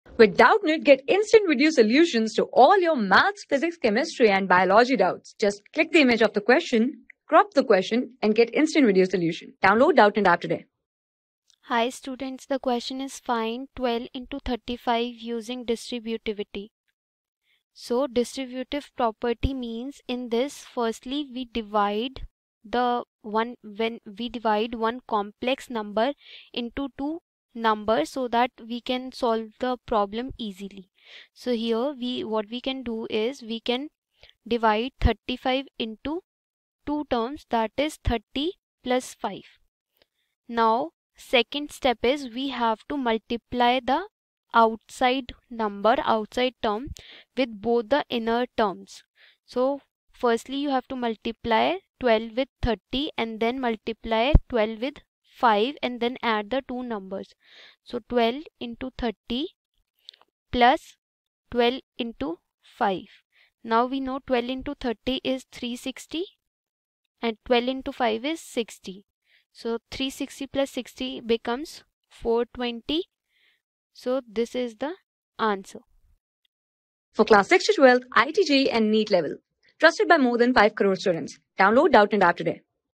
Find 12xx35 using distributivity | 6 | WHOLE NUMBERS | MATHS | NCERT ENGLISH | Doubtnut
Find 12xx35 using distributivity
Class: 6
Subject: MATHS
Chapter: WHOLE NUMBERS
Board:CBSE

👉 Have Any Query? Ask Us.

Our Telegram Pages:
class 6
6 class ka math syllabus